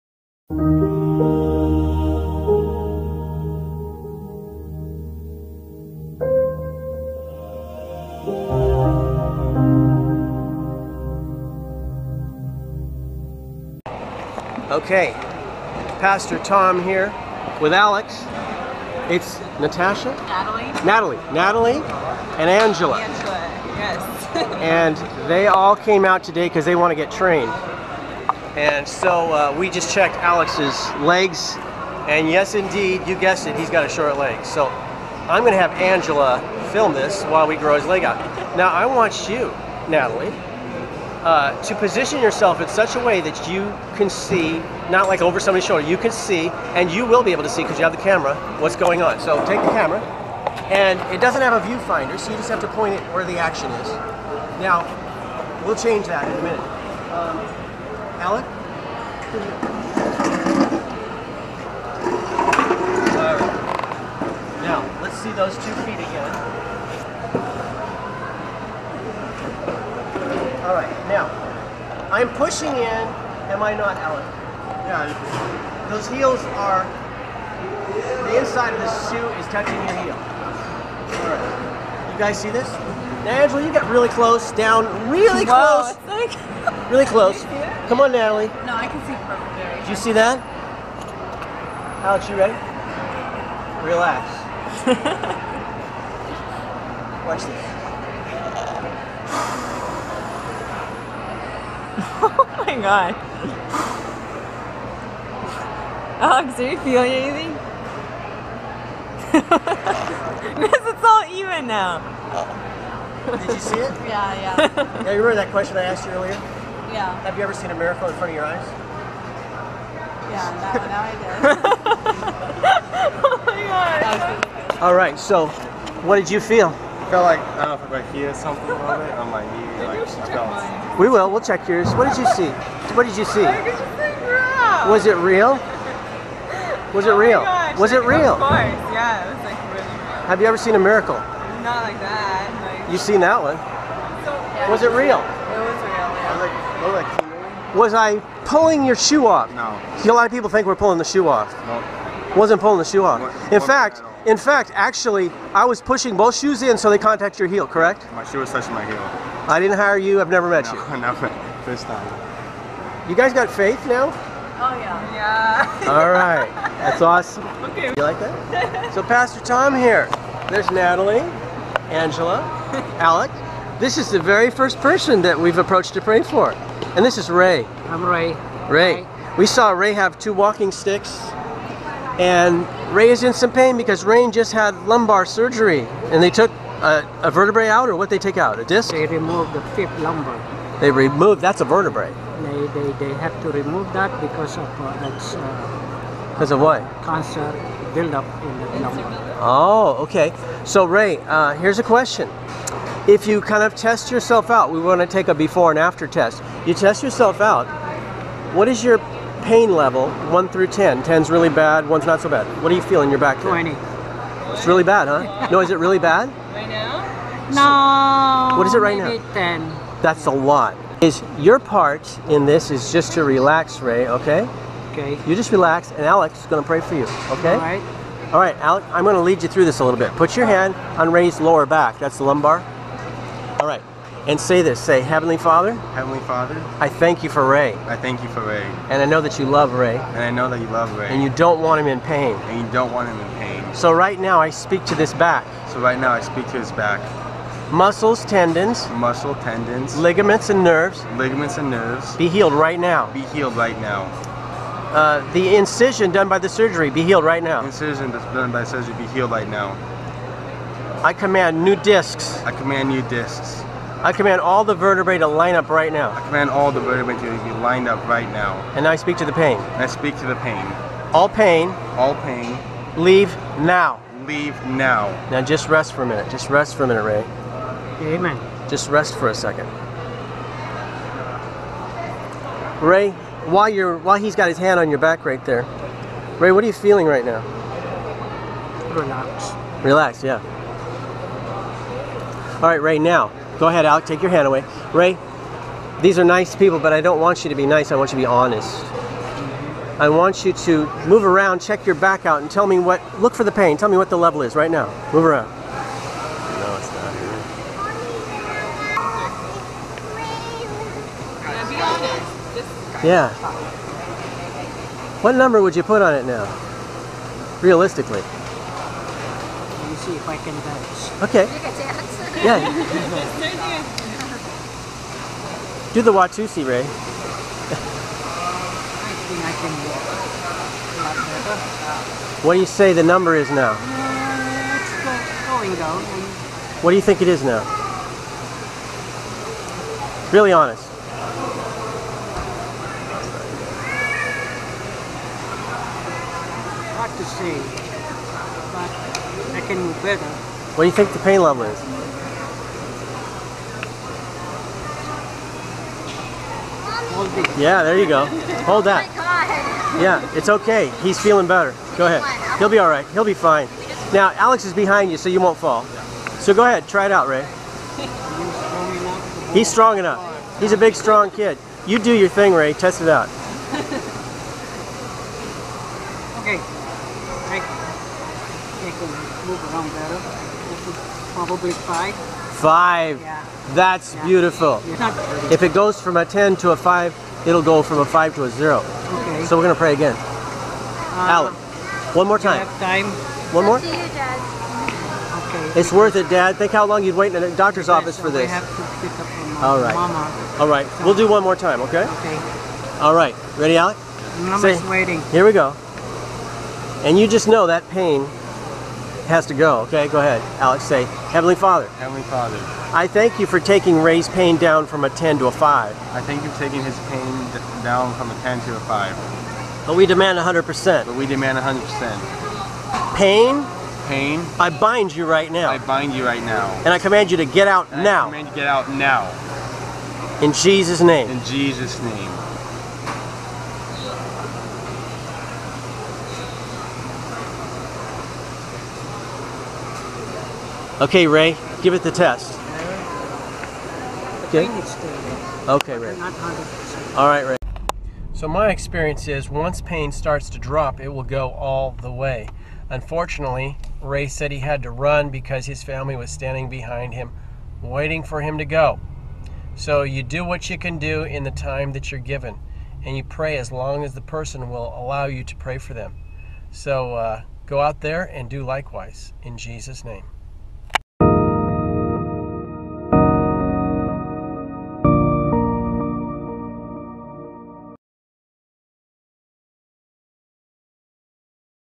Novi učenci vidijo prvič čudeže v živo!
Zdravljenje v živo, poglejte si še ostala njegova zdravljenja.

This group of young Ukrainians wanted to go out and train, of course I said YES. Every believer has what he needs to walk in miracles signs and wonders, you just need to know how to use it.